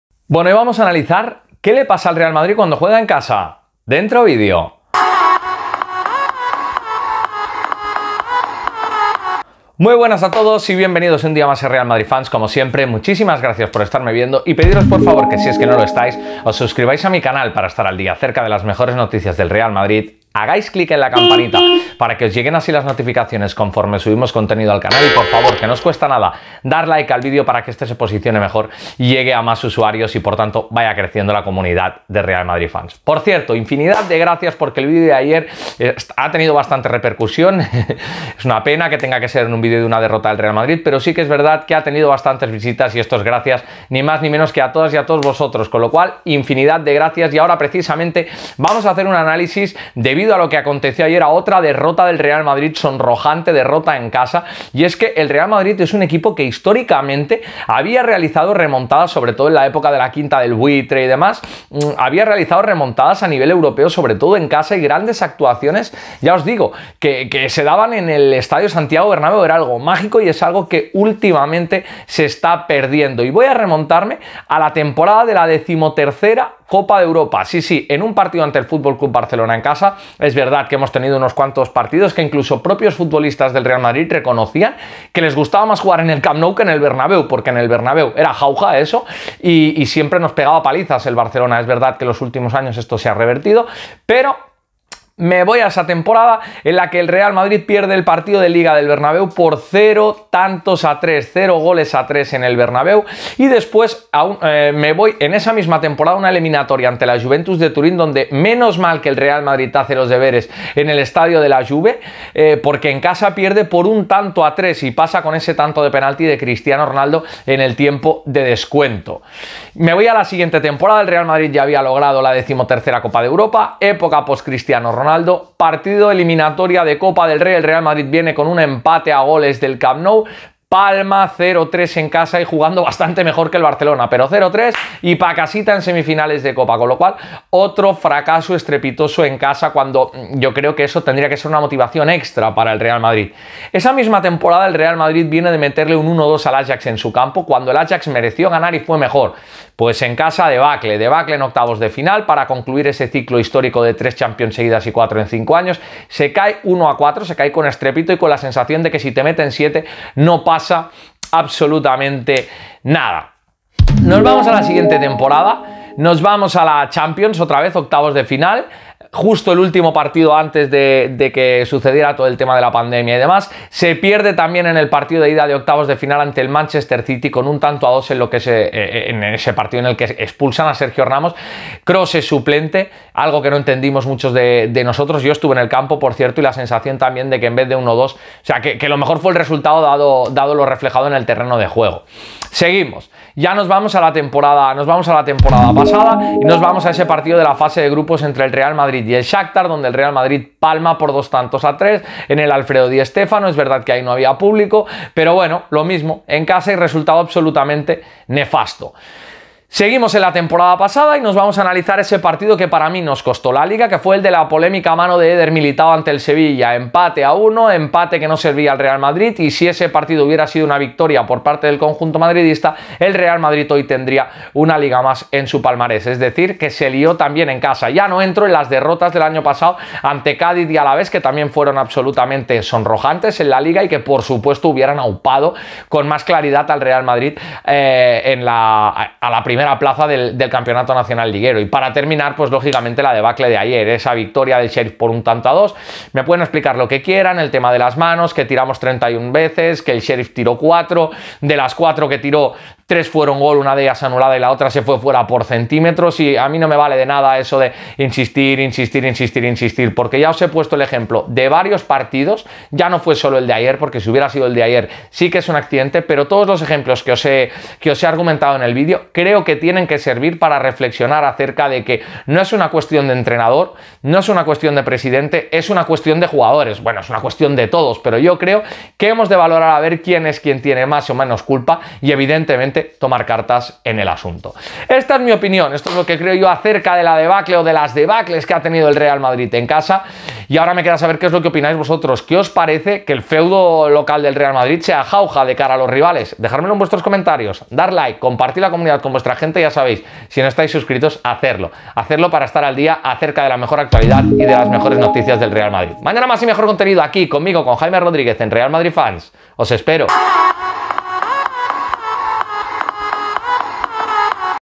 🤔¿Por qué pierde tanto jugando en casa el REAL MADRID?💯
En Real Madrid Fans analizamos los partidos más importantes que ha perdido el Real Madrid jugando en casa y lo que ha implicado cada una de sus derrotas.